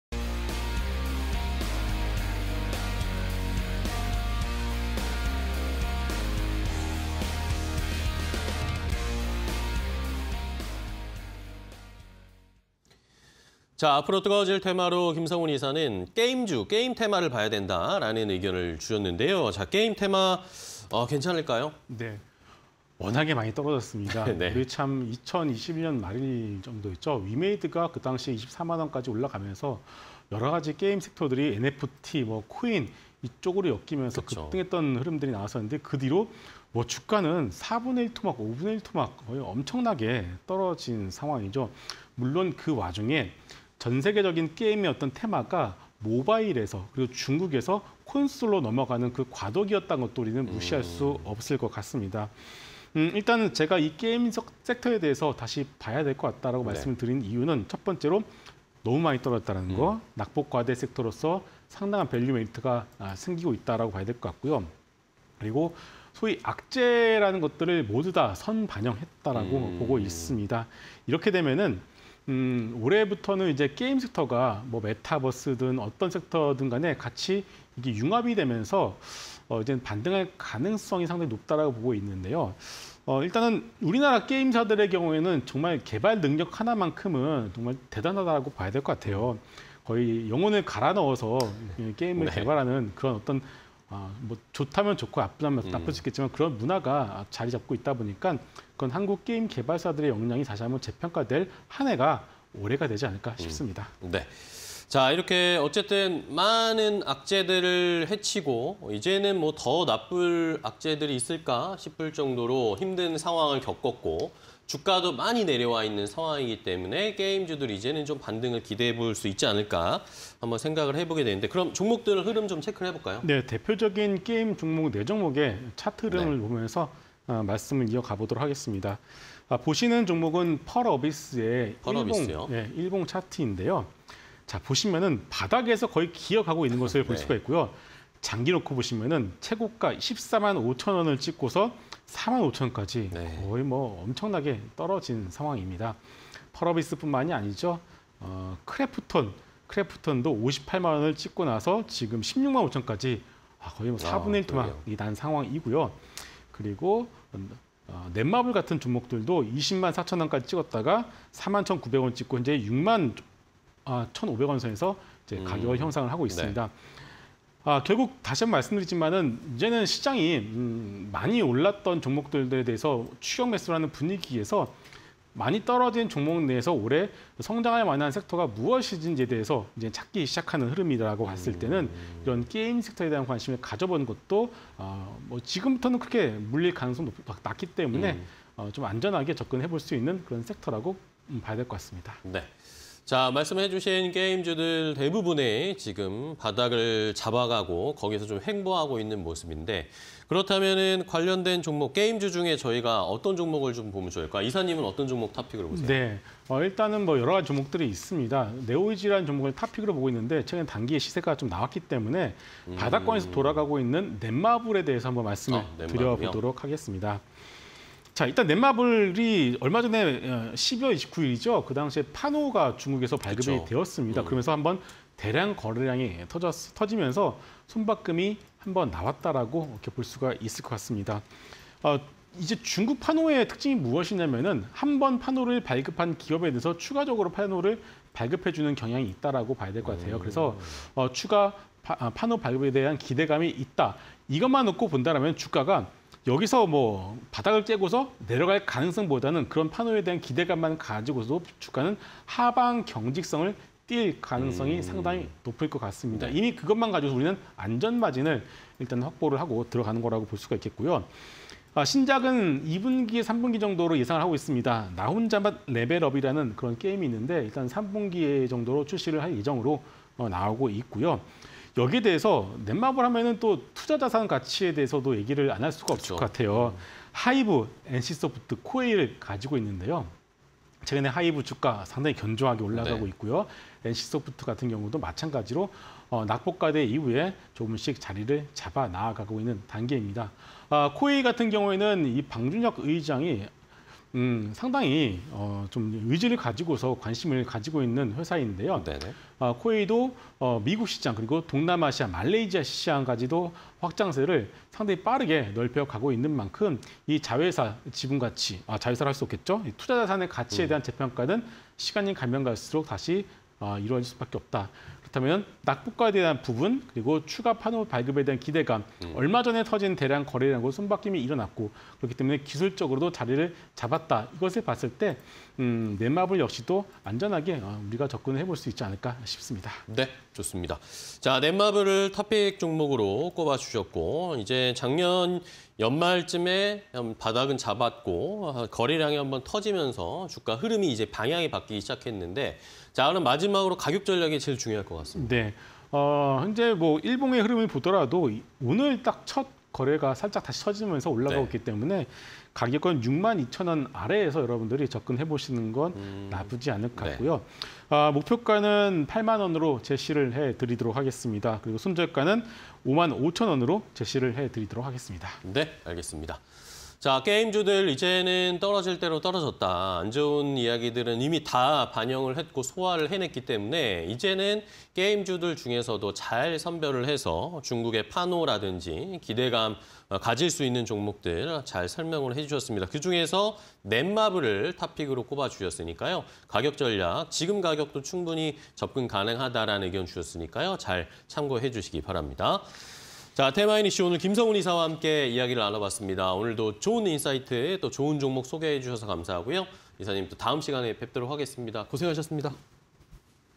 넷마블(251270)... 게임주_테마IN이슈 (20230126)
- 낙폭과대 섹터… '부담' 적어
- 대부분 악재 선반영 판단
- 한국 게임 개발 능력 '재조명' 필요
- 펄어비스, 크래프톤, 넷마블 , 네오위즈

넷마블(251270)
- 신작 기대감, 현재 주가 '바닥' 지지
- 中 추가 판호 발급 기대감
- 3분기 신작 '나혼자만 레벨업' 기대감
- 투자자산 가치 재평가… 하이브ㆍ엔씨소프트 등